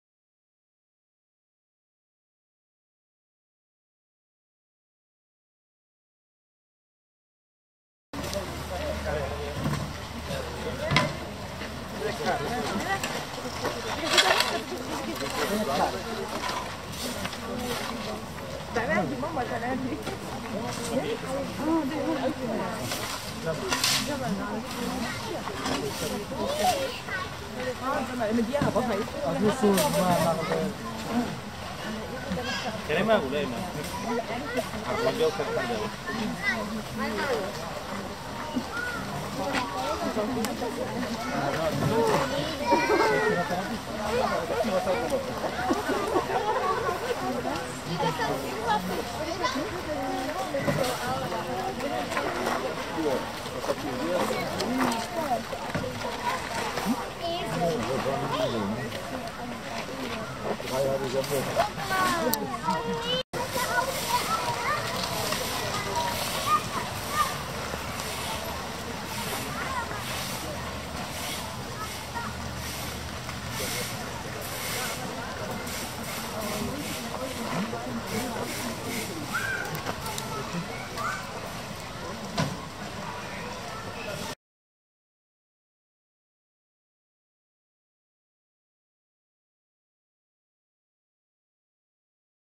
Onager
Animals in the Tierpark Hagenbeck (Hamburg)
Only animals and nothing else :)